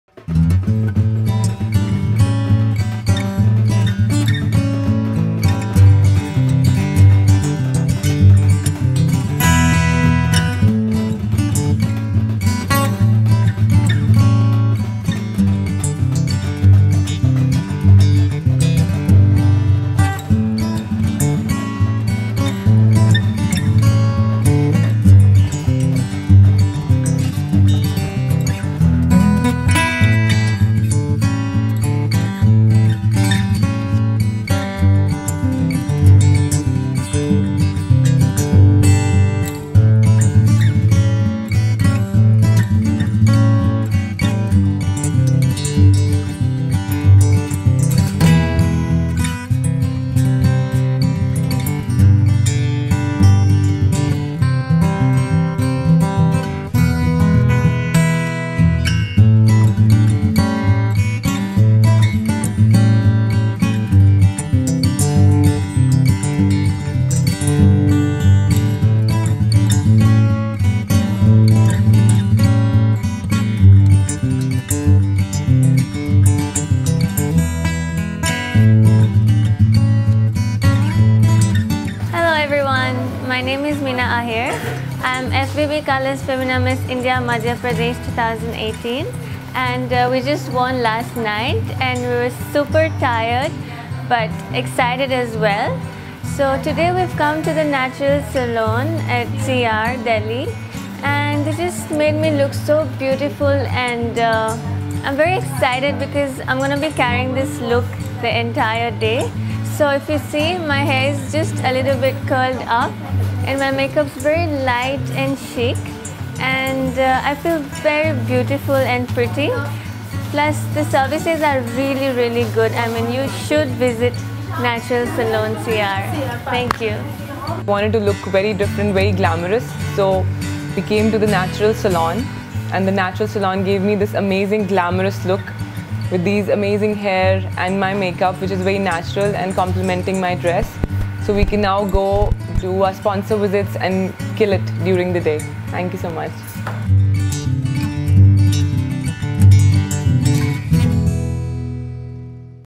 Miss India North 2018 finalist visit Naturals store in Delhi
When fbb Colors Femina Miss India North finalist visited Naturals store in Delhi.